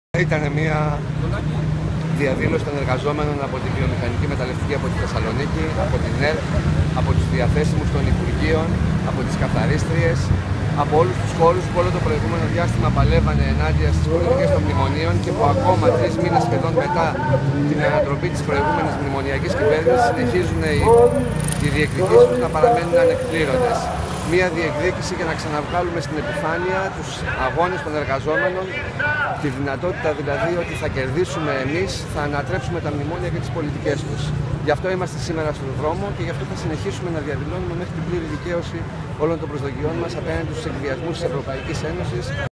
Δήλωση του Τ. Αναστασιάδη μετά τη συγκέντρωση στο υπ. Εργασίας
Η δήλωση του Τάσου Αναστασιάδη μετά το πέρας της συγκέντρωσης έξω από το υπουργείο Εργασίας που πραγματοποίησαν σωματεία εργαζομένων που τέθηκαν σε διαθεσιμότητα ή απολύθηκαν.